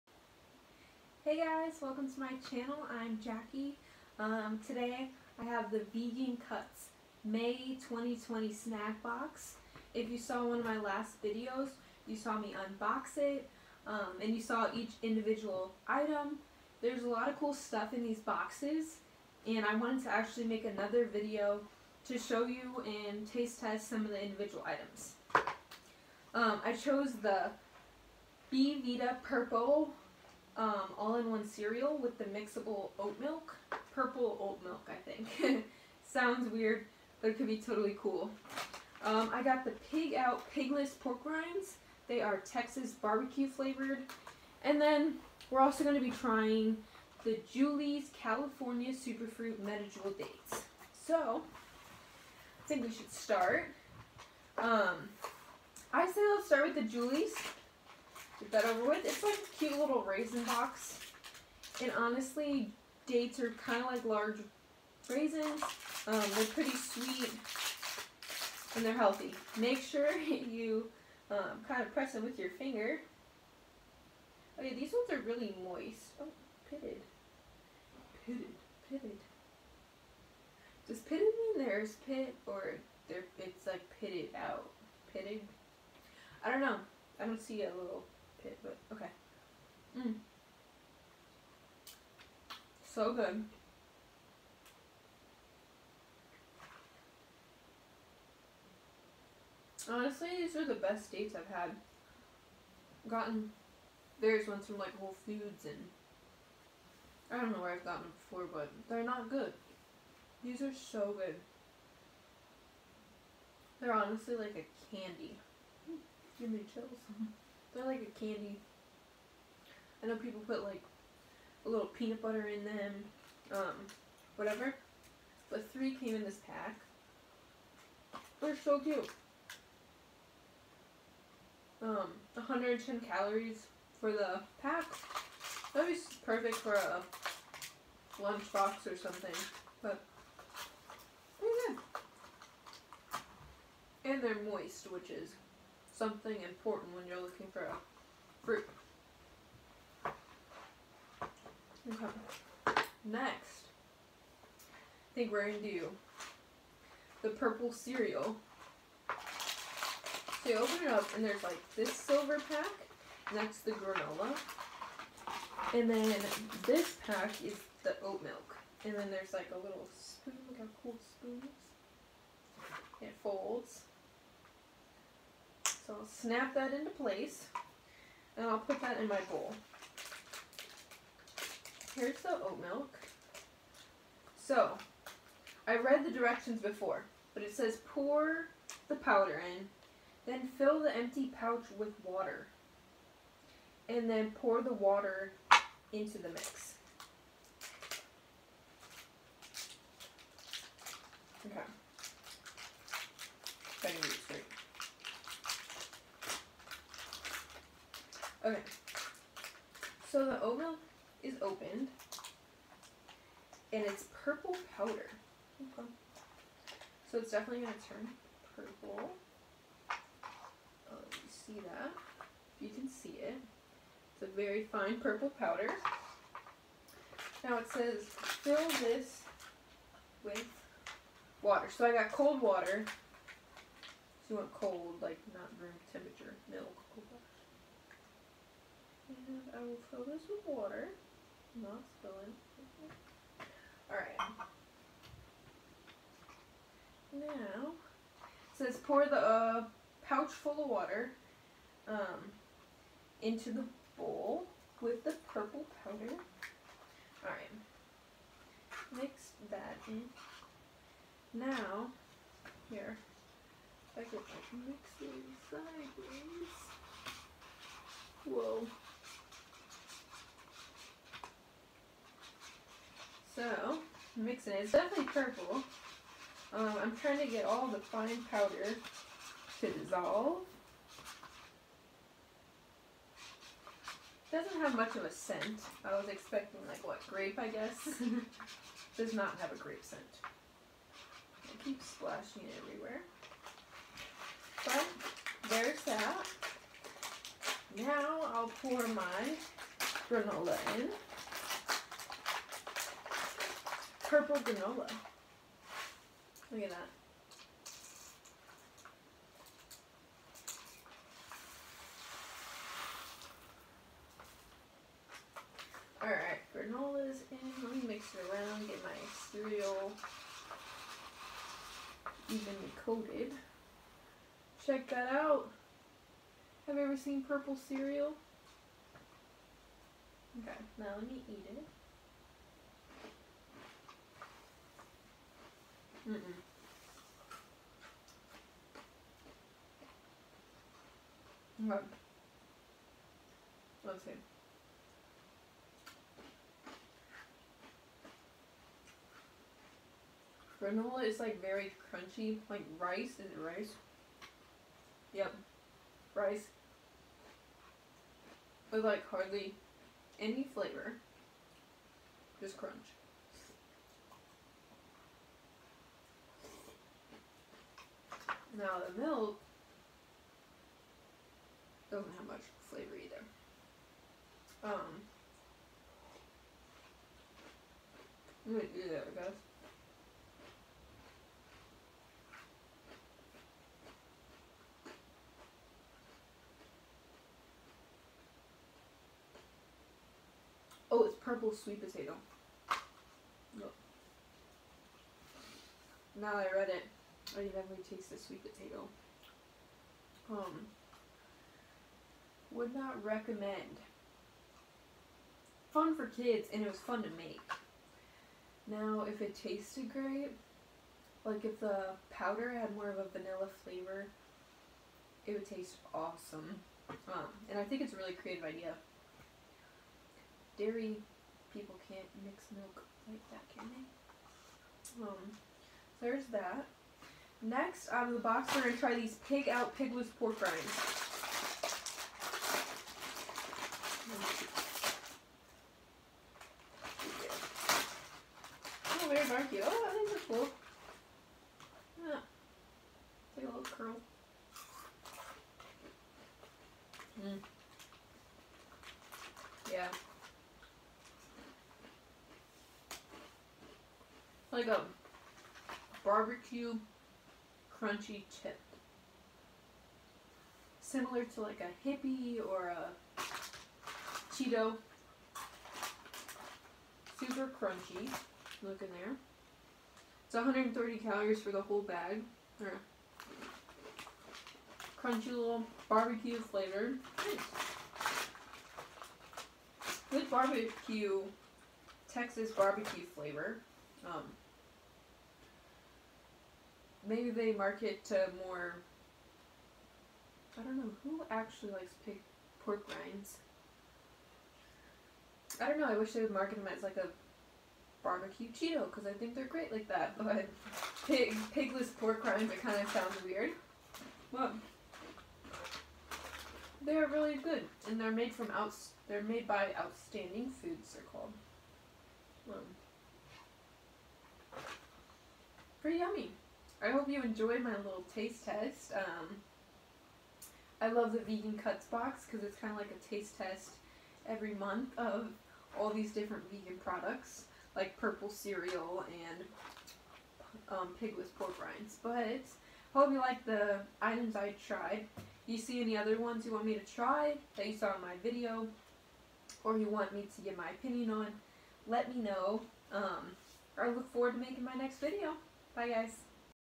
VeganCuts taste test May 2020 snack box
Trying out items from the May 2020 VeganCuts snack subscription box! Yum! Lots of vegan snacks, like granola cereal, porkless rinds, and dates! Yummy vegan snack options!